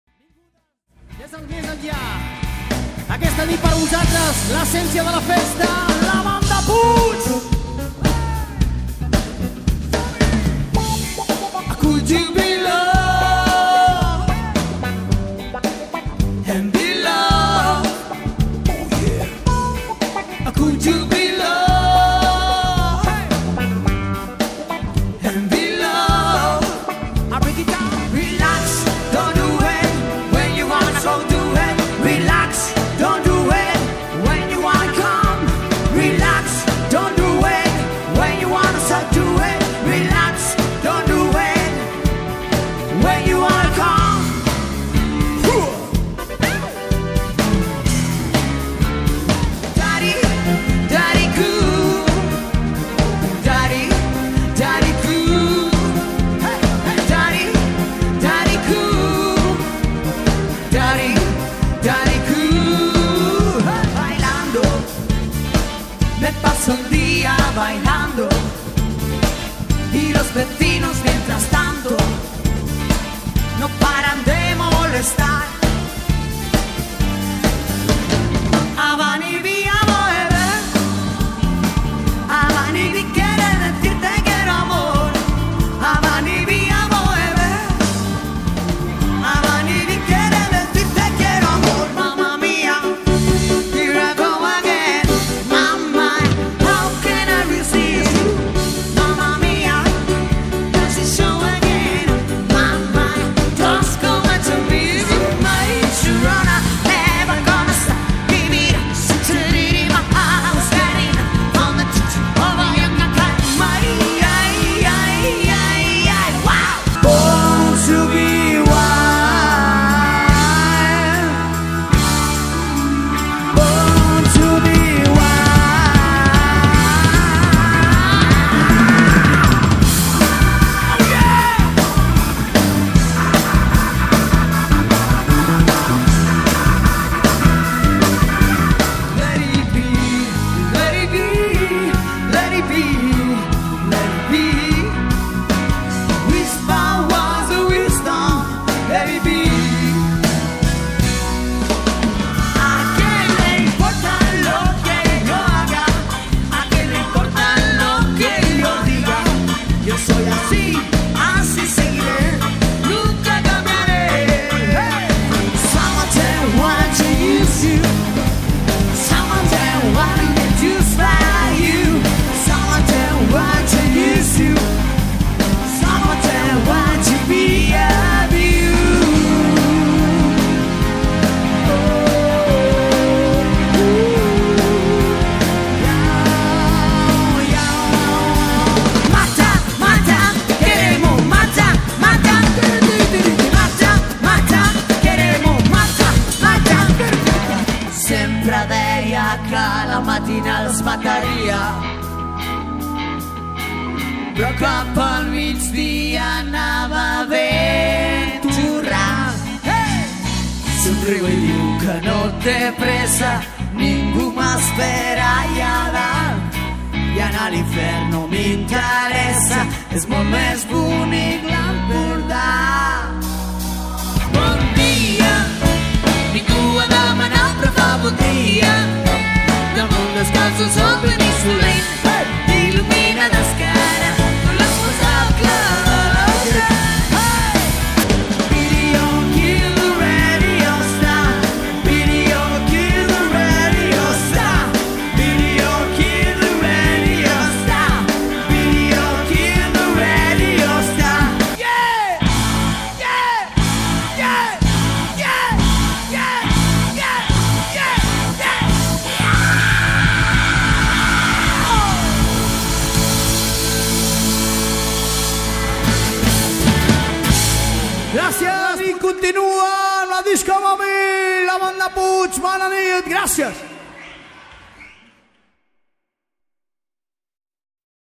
LA BANDA PUIG. VIDEO PROMOCIONAL GIRA 2010/2011
GRUPO DE VESIONES MULTIGENERACIONAL CON MAS DE 10 AÑOS DE EXPERIENCIA SOBRE LOS ESCENARIOS. MAS DE 70 CANCIONES DURANTE 2,5H. DE SHOW FIESTERO. LOS 4X4 DE LOS ESCENARIOS. LA ESENCIA DE LA FIESTA.